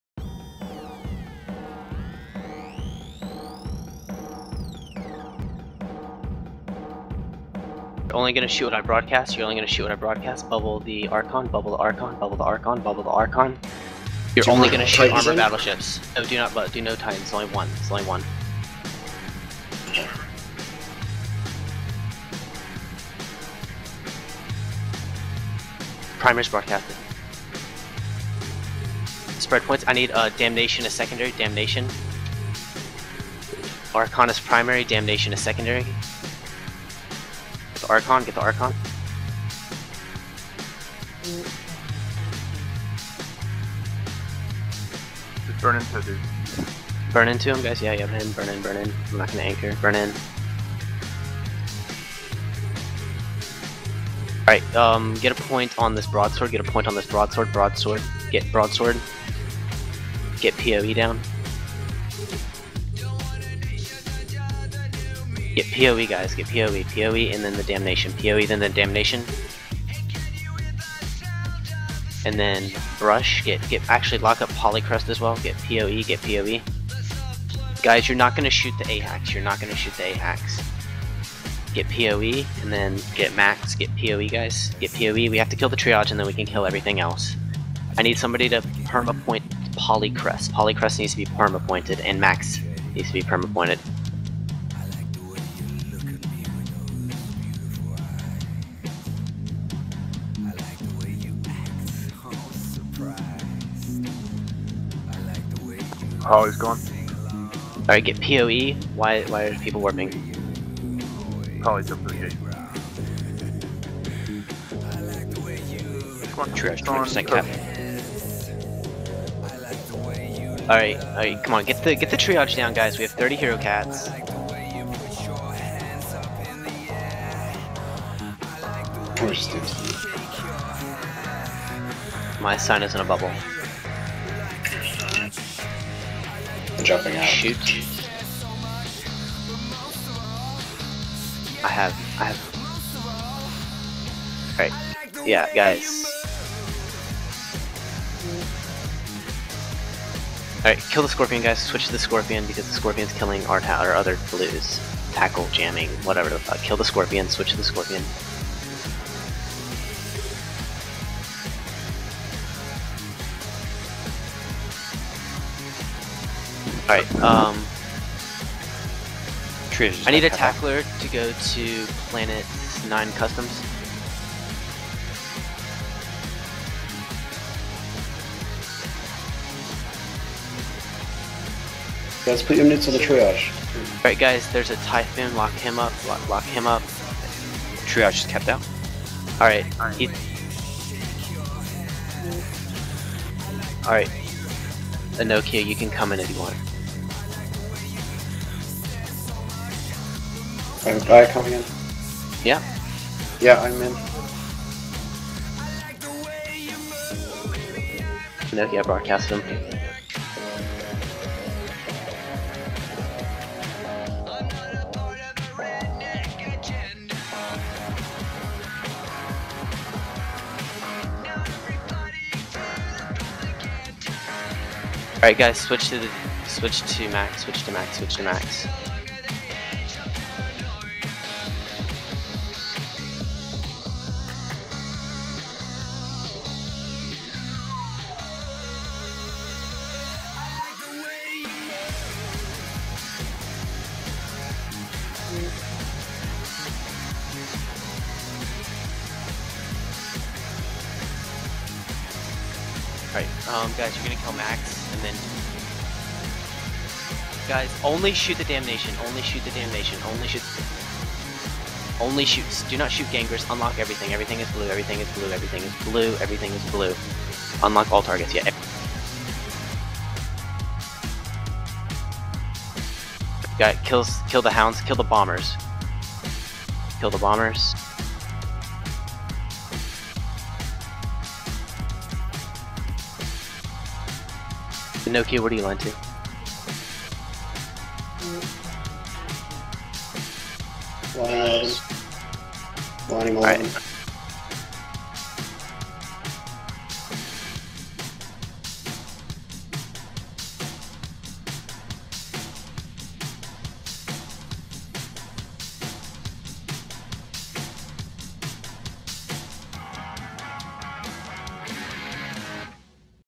2012.11.11 in M-MD3B - NCdot hotdropping in Great Wildlands
NCdot hotdrops a herocat fleet onto an armor fleet and kills their one triage carrier along with assorted support ships.